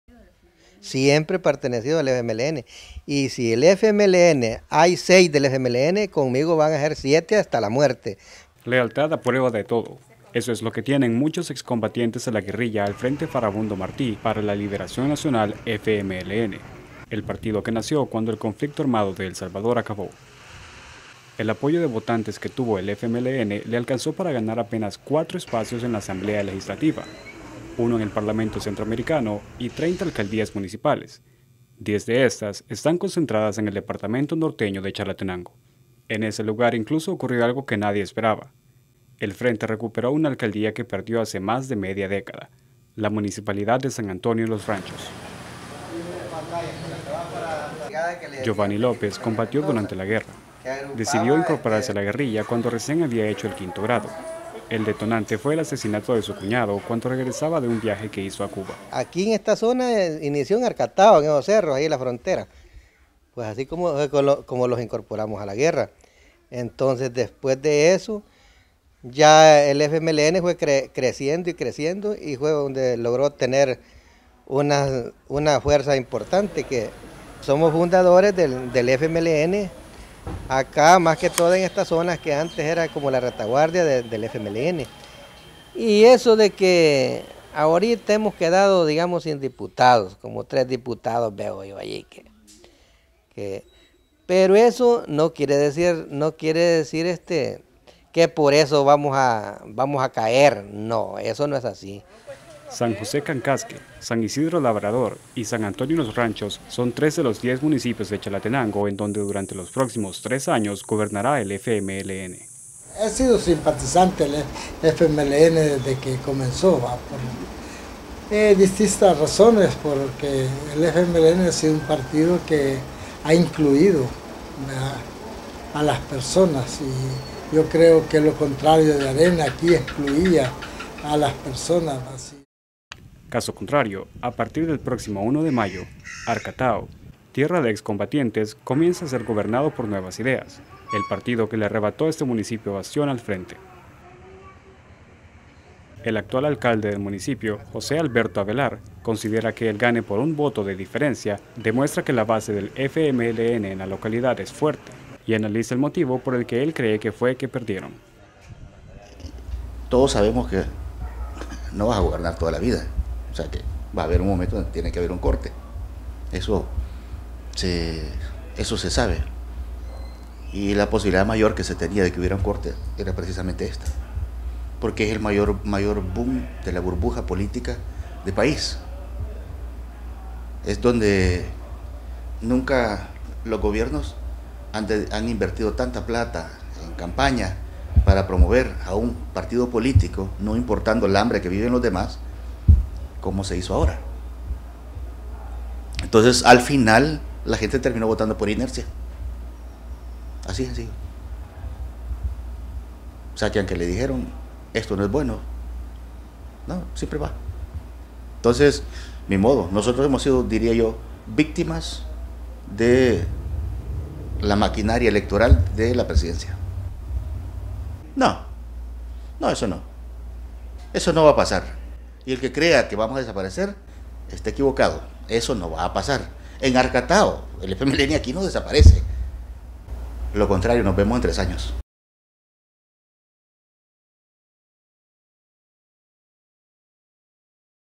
Los últimos rebeldes del FMLN
El apoyo de votantes que tuvo el FMLN le alcanzó para ganar apenas cuatro espacios en la Asamblea Legislativa, uno en el Parlamento Centroamericano (PARLACEN) y 30 alcaldías municipales, 10 de éstas, están concentradas en el departamento norteño de Chalatenango.